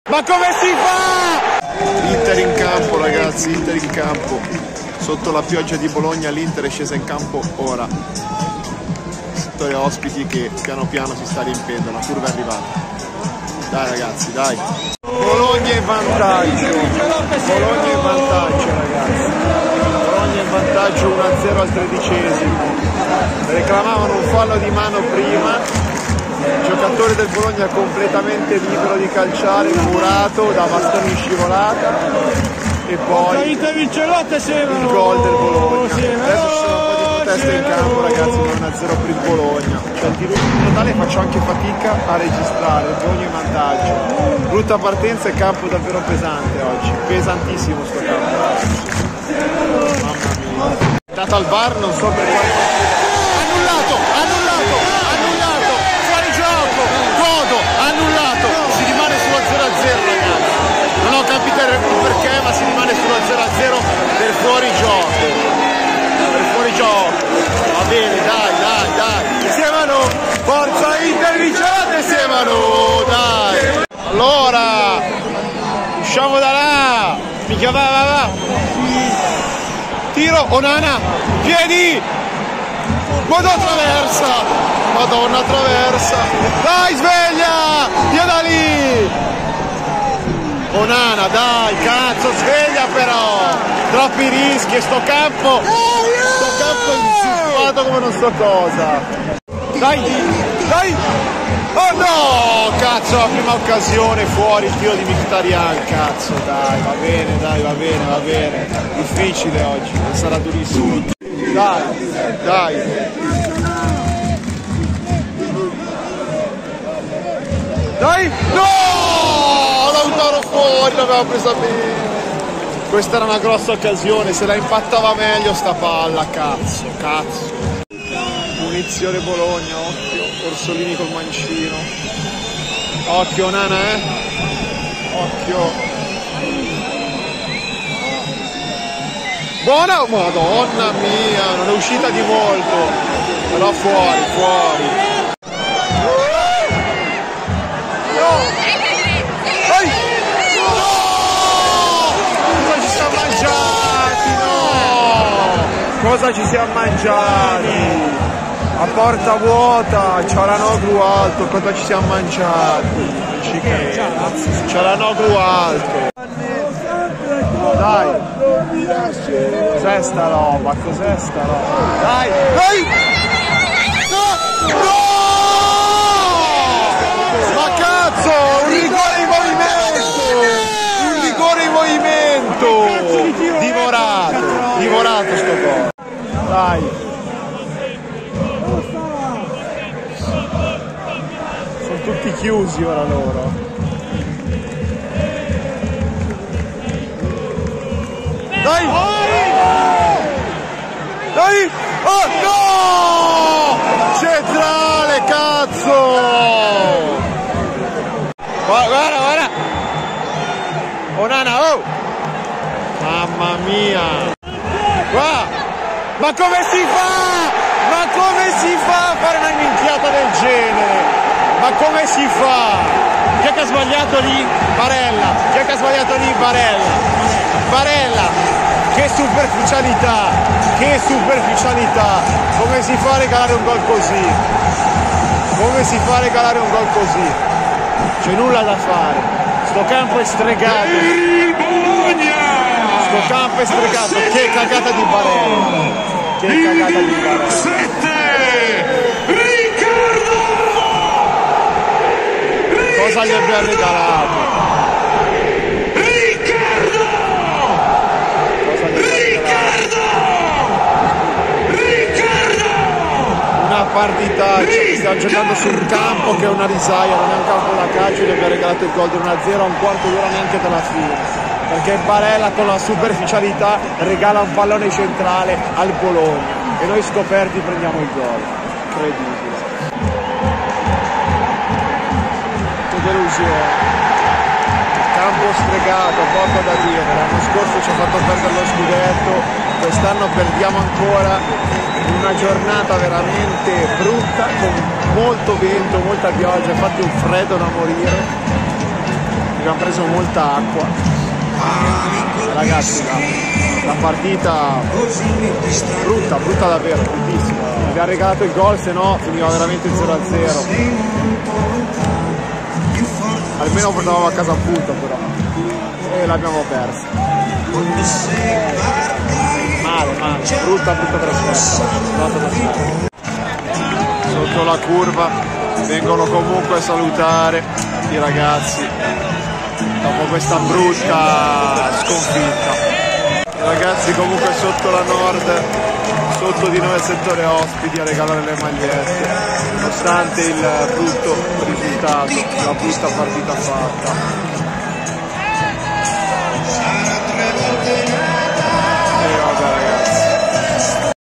IMBARAZZANTI! BOLOGNA-INTER 1-0 | LIVE REACTION TIFOSO INTERISTA DAL SETTORE OSPITI DEL DALL'ARA
🔴Bologna-Inter 1-0 (LIVE REACTION)
🏆24' Giornata
🏟️Renato Dall'Ara
🕘12:30
⚽Gol Bologna 1-0 minuto 3:36
📲Seguitemi anche su Instagram: